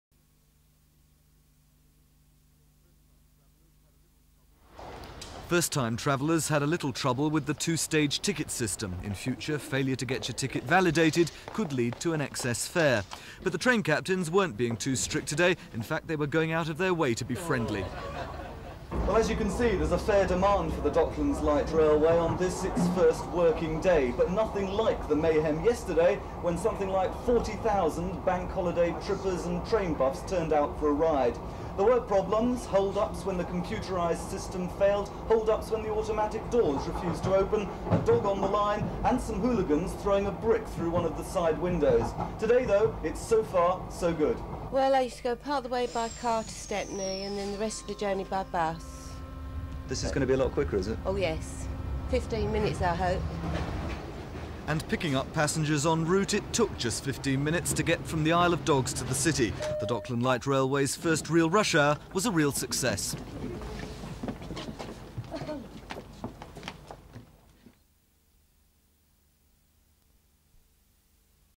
Docklands Light Railway | new ticketing system | East London | DLR | 1987
Docklands Light railways new ticketing system.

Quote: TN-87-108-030